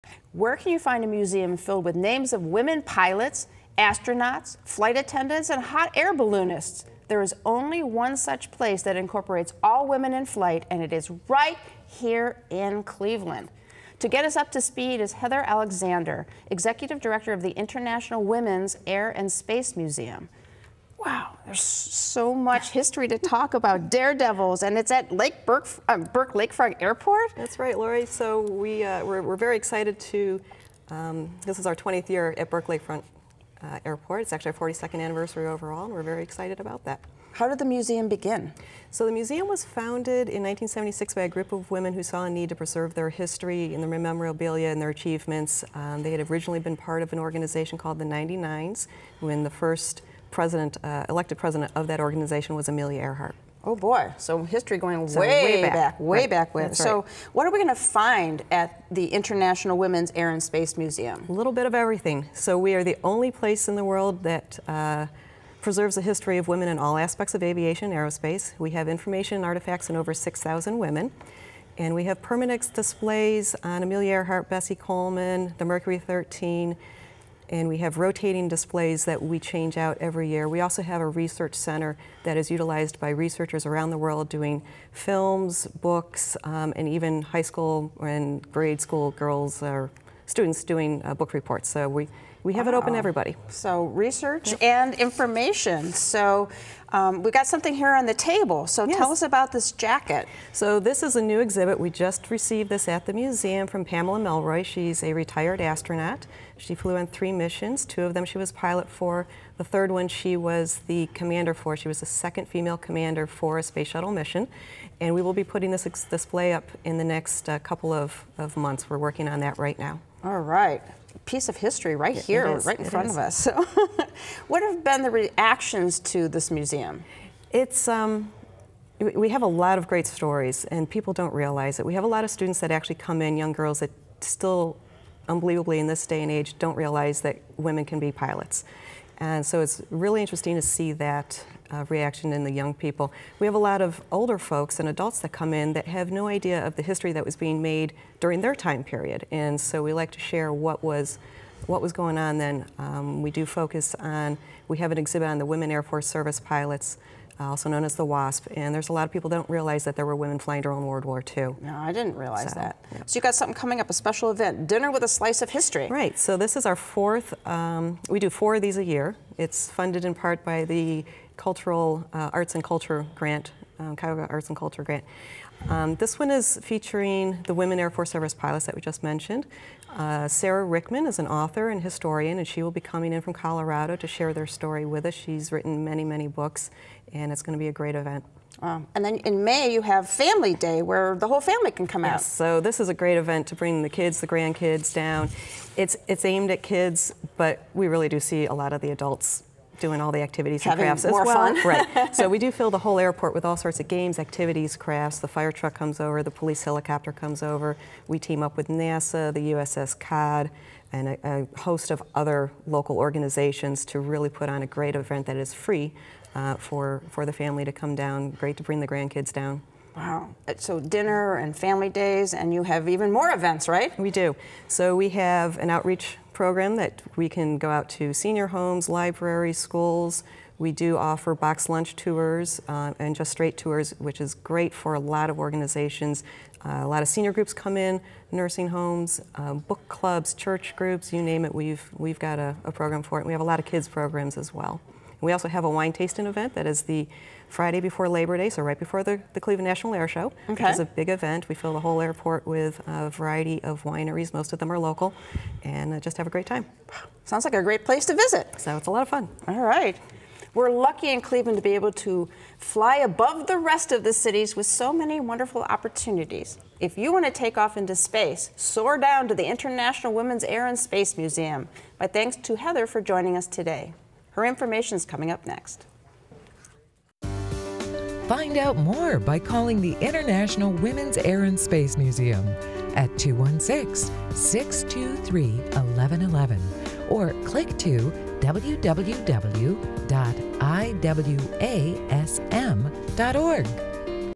Golden Opportunities International Women’s Air & Space Museum 4/22/18 show #888
We’ll lift off with women in flight.